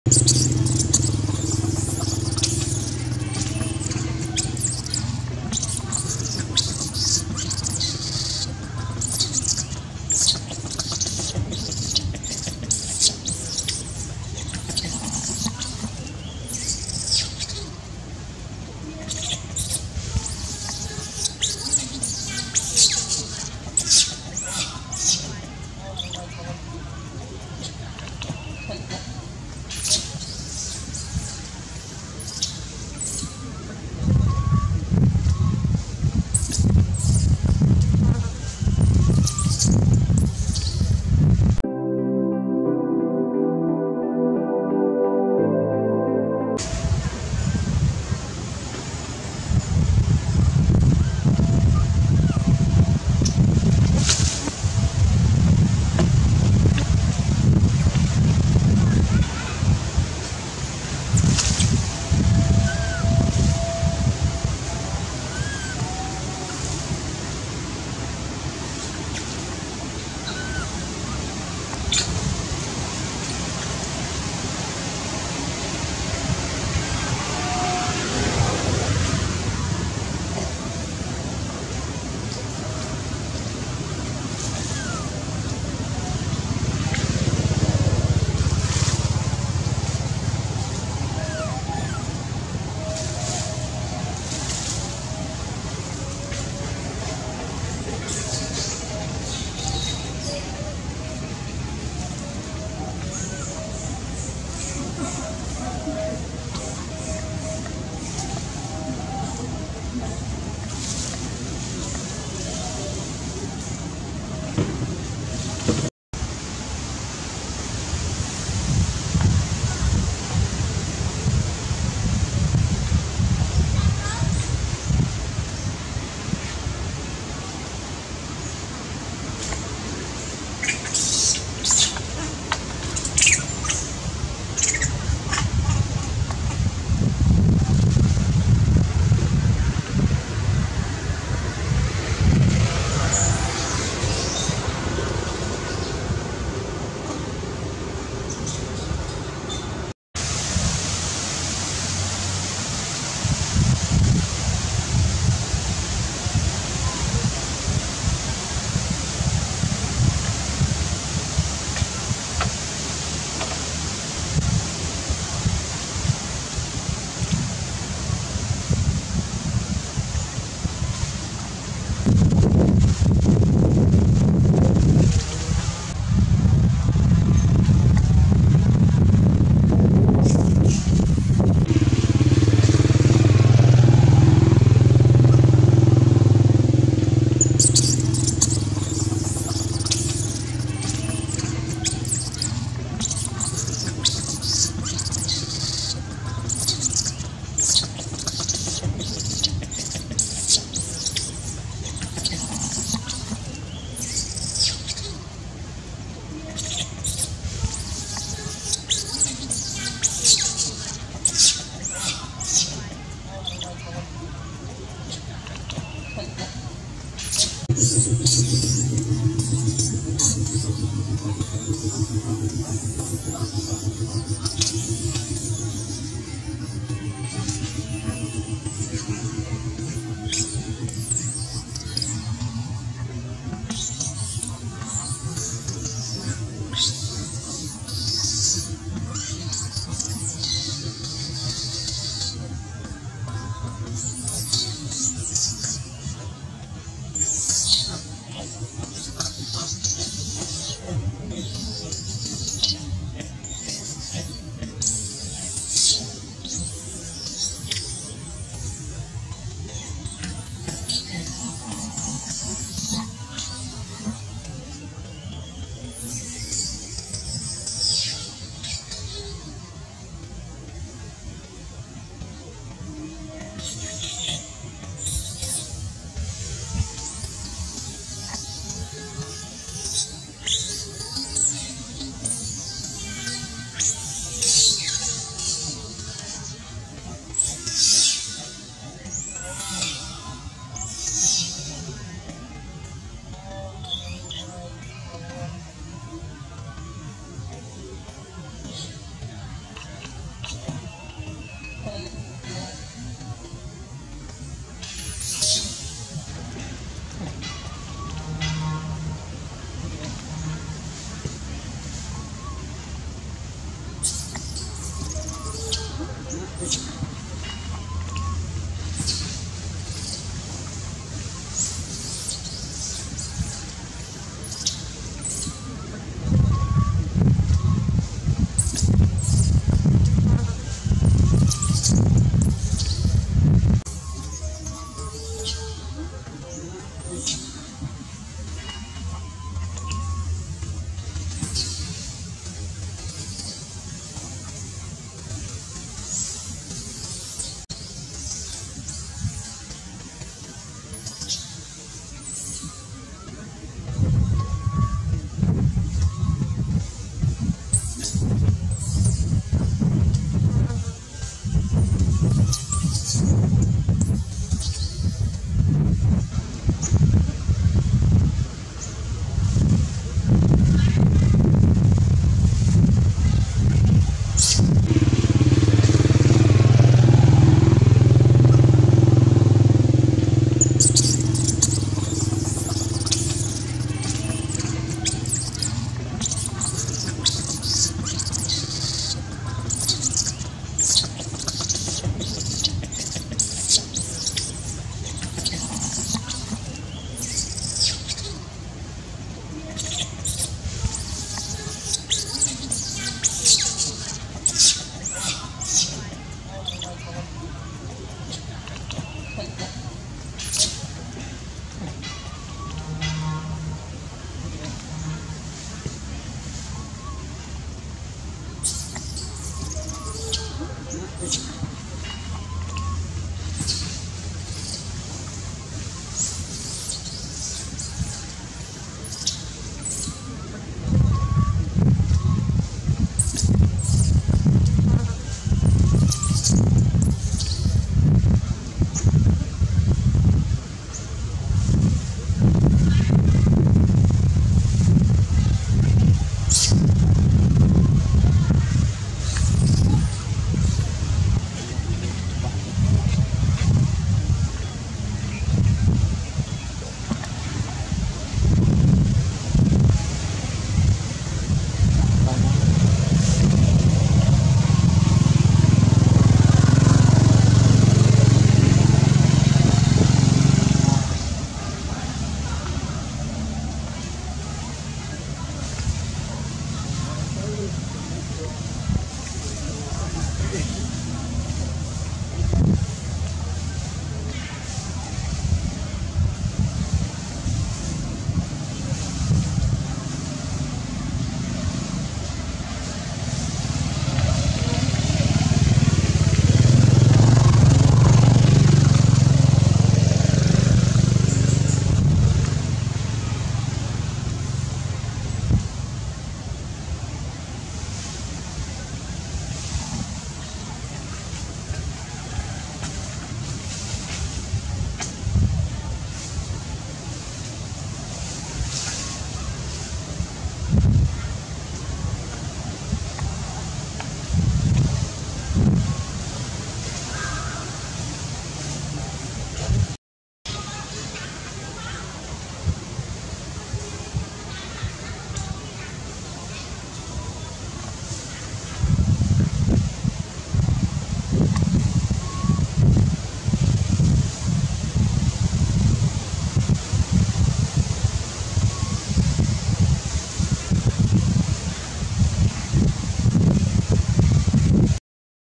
Super clip! Extremely angry, Mom fight own baby even if other one can't help, Pity baby cries loudly
hello! every body that watching my videos. Now I want to show new update : Super clip! Extremely angry, Mom fight own baby even if other one can't help, Pity baby cries loudly